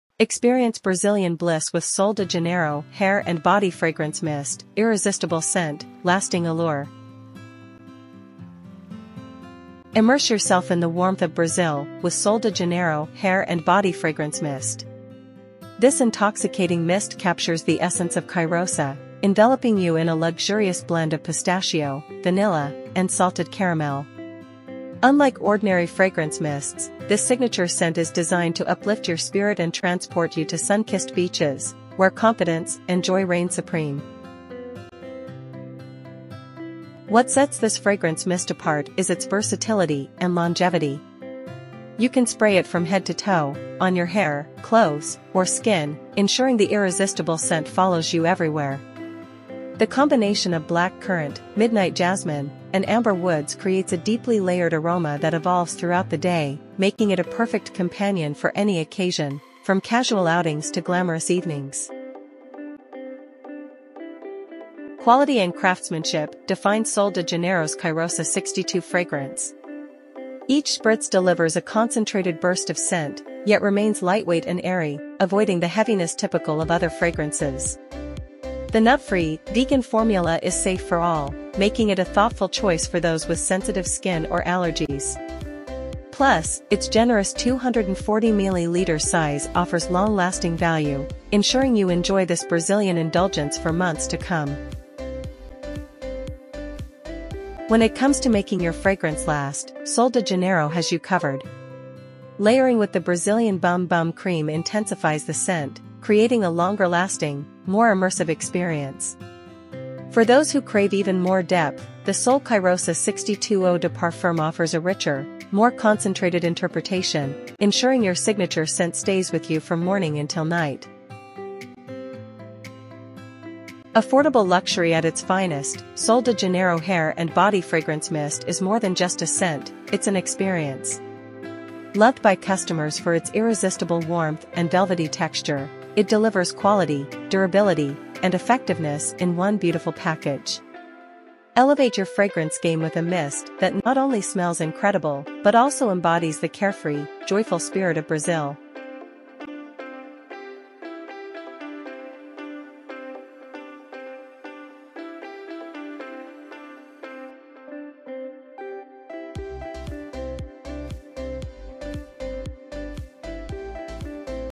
Experience Brazilian Bliss with SOL DE JANEIRO Hair & Body Fragrance Mist – Irresistible Scent
Immerse yourself in the warmth of Brazil with SOL DE JANEIRO Hair & Body Fragrance Mist. This intoxicating mist captures the essence of cheirosa, enveloping you in a luxurious blend of pistachio, vanilla, and salted caramel. Unlike ordinary fragrance mists, this signature scent is designed to uplift your spirit and transport you to sun-kissed beaches, where confidence and joy reign supreme.

What sets this fragrance mist apart is its versatility and longevity. You can spray it from head to toe, on your hair, clothes, or skin, ensuring the irresistible scent follows you everywhere. The combination of black currant, midnight jasmine, and amber woods creates a deeply layered aroma that evolves throughout the day, making it a perfect companion for any occasion, from casual outings to glamorous evenings.

Quality and craftsmanship define SOL DE JANEIRO’s Cheirosa 62 fragrance. Each spritz delivers a concentrated burst of scent, yet remains lightweight and airy, avoiding the heaviness typical of other fragrances. The nut-free, vegan formula is safe for all, making it a thoughtful choice for those with sensitive skin or allergies. Plus, its generous 240mL size offers long-lasting value, ensuring you enjoy this Brazilian indulgence for months to come.

When it comes to making your fragrance last, SOL DE JANEIRO has you covered. Layering with the Brazilian Bum Bum Cream intensifies the scent, creating a longer-lasting, more immersive experience. For those who crave even more depth, the Sol Cheirosa 62 Eau de Parfum offers a richer, more concentrated interpretation, ensuring your signature scent stays with you from morning until night.

Affordable luxury at its finest, SOL DE JANEIRO Hair & Body Fragrance Mist is more than just a scent—it's an experience. Loved by customers for its irresistible warmth and velvety texture, it delivers quality, durability, and effectiveness in one beautiful package. Elevate your fragrance game with a mist that not only smells incredible but also embodies the carefree, joyful spirit of Brazil.